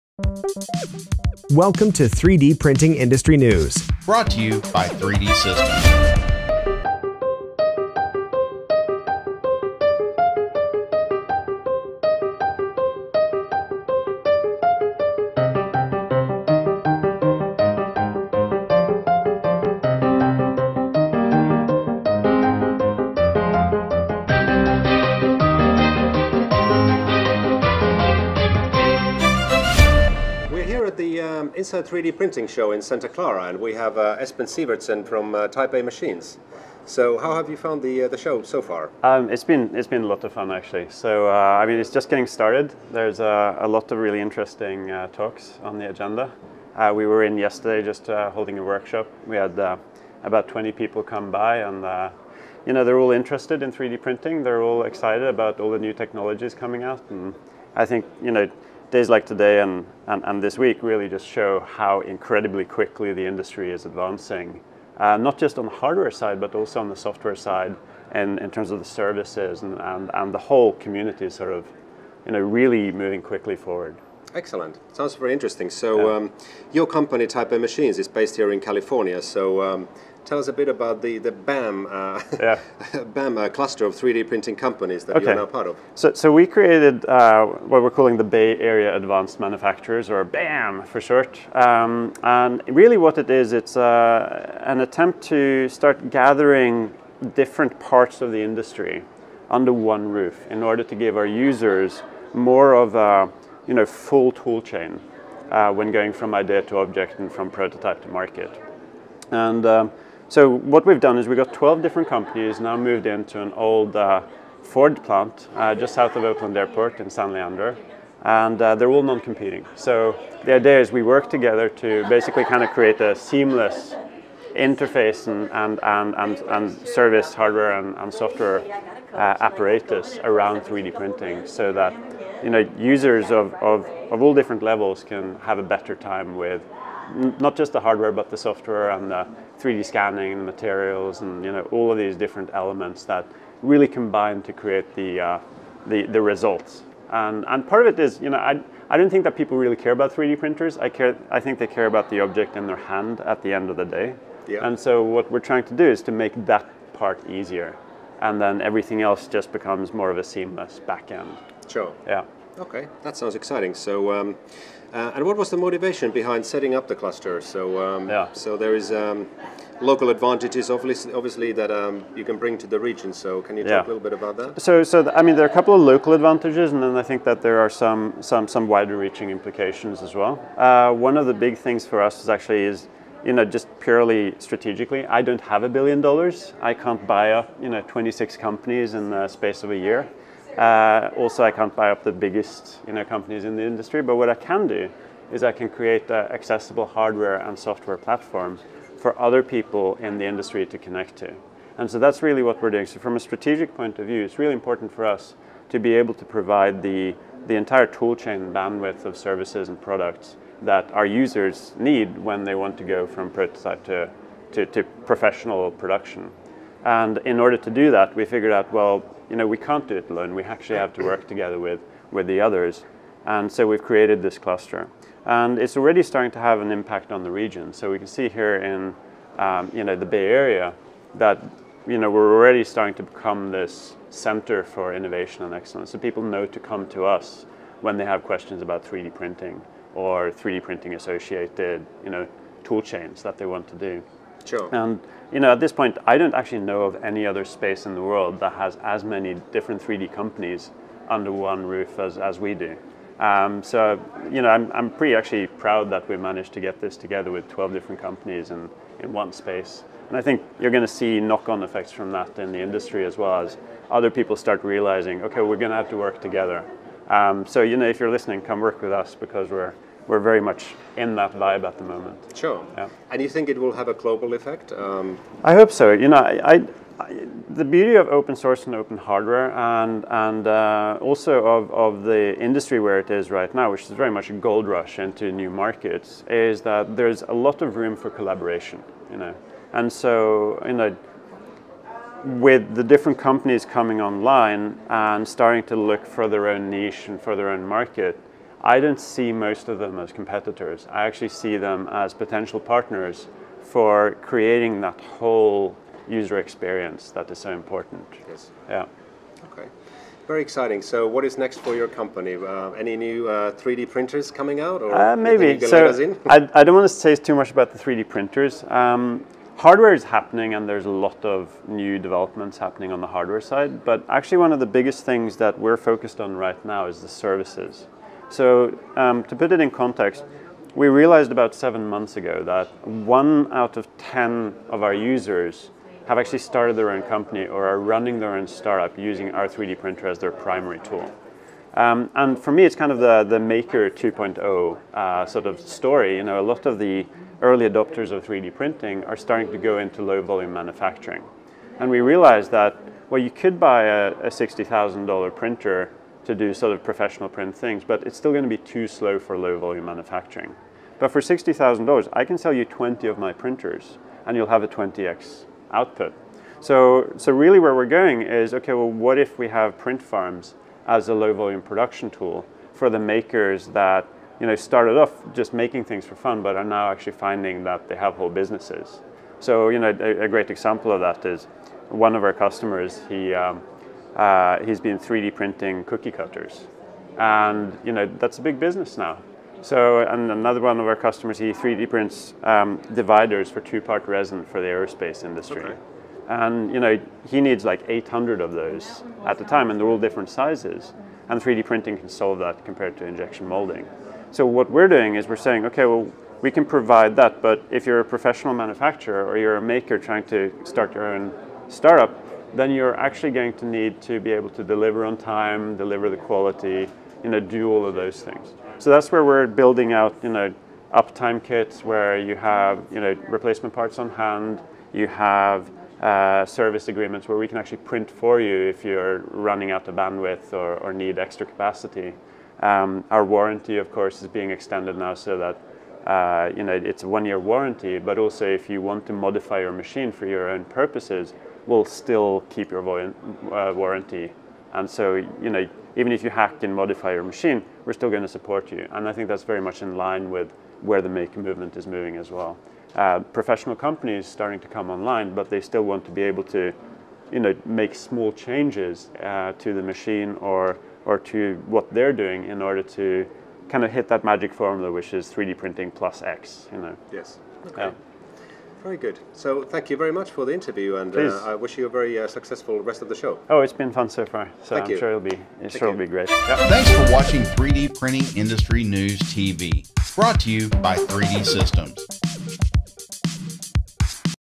Interview with Mr Espen Sivertsen from Type A Machines
At the recent Inside 3D Printing Conference in Santa Clara, 3DPI.TV Interviewed Espen Sivertsen, the CEO and Co-Founder of Type A Machines, the manufacturer of Series 1 3D Printers.We talked about the Bay Area Advance Manufacturers (BAAM) which gathers different parts of the 3D Printing Industry under one roof.